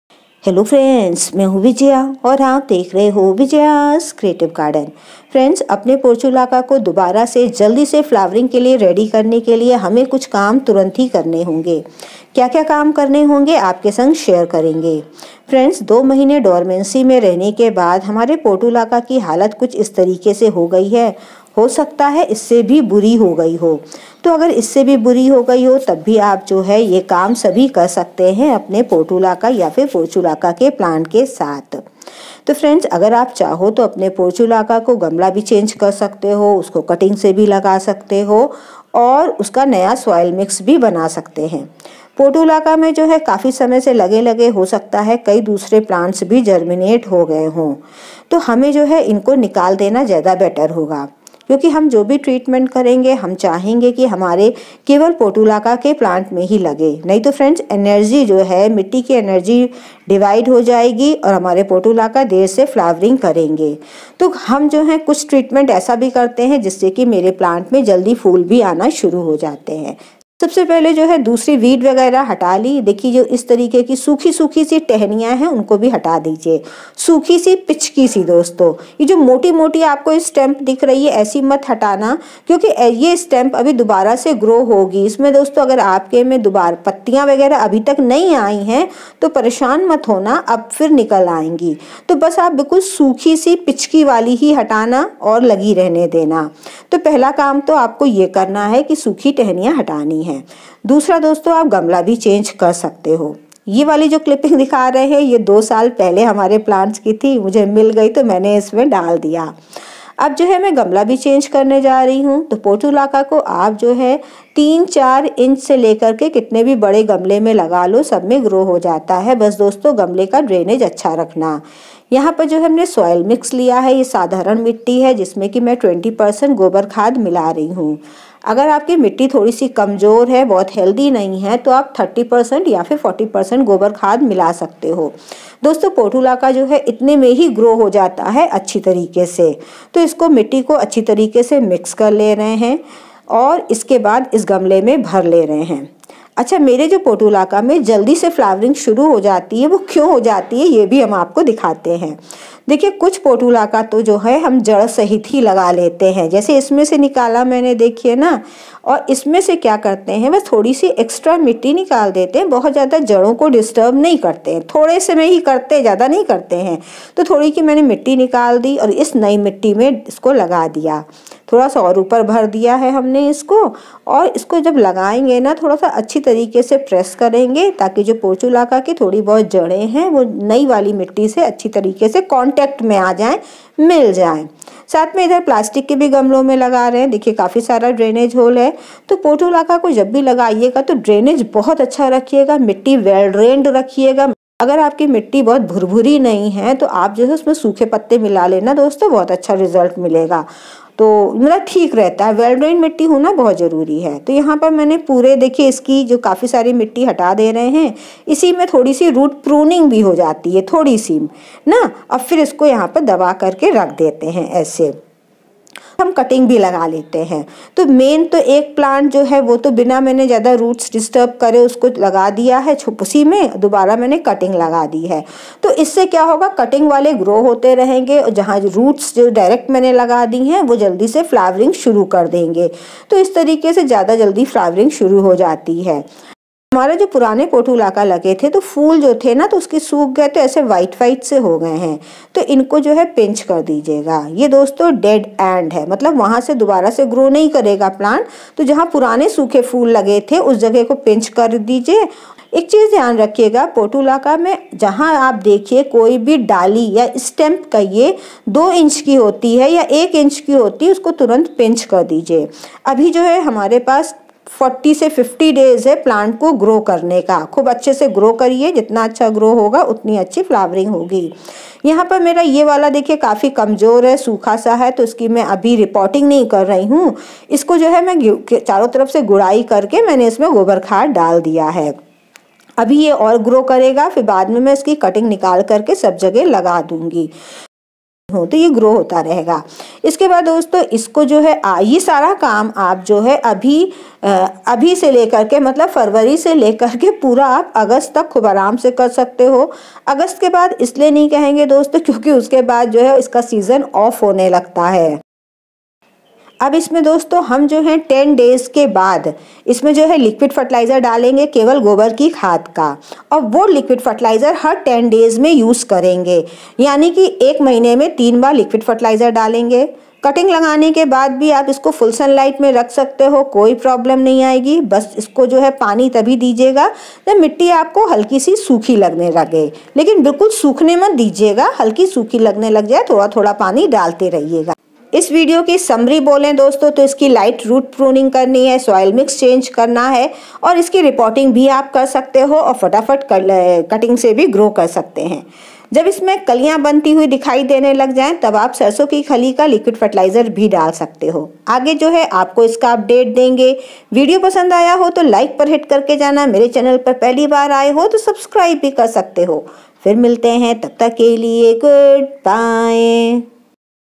Portulaca को फूलो के लिऐ तैयार करे 3 steps मे || Make portulaca/ Moss rose ready for flowering
portulaca ko phoolon ke liye taiyar kariye three steps.make portulaca or moss rose ready for flowering.spring gardening.office time